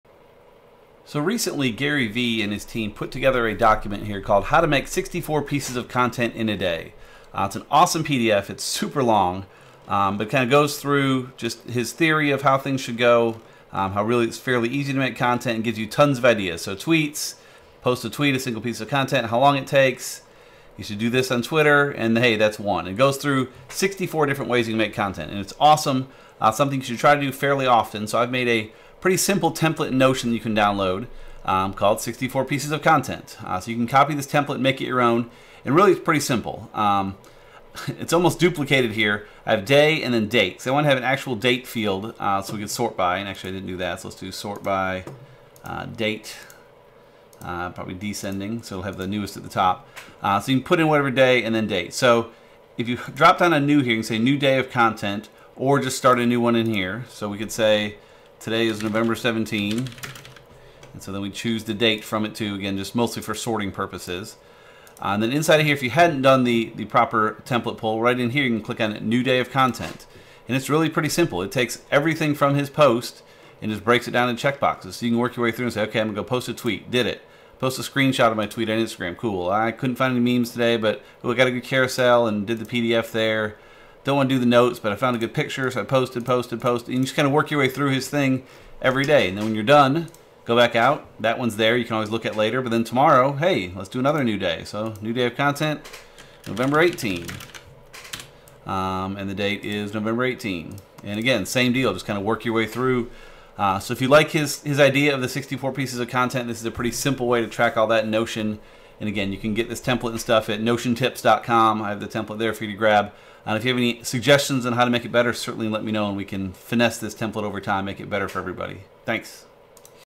Creating 64 pieces of content in a day with Notion
If you want to learn more about Notion, I strongly recommend you take a look at Marie or Francesco's notion course. They've both done incredible things with Notion, and you will learn a ton from either one.

Gary Vee recently put out a great post (and corresponding PDF) with 64 ways to create content in a day. I strongly encourage you to check out his work here:

If you plan to work through those 64 pieces of content daily (or even from time to time), this Notion template will help you easily track your progress. You can grab the Notion template on our site here: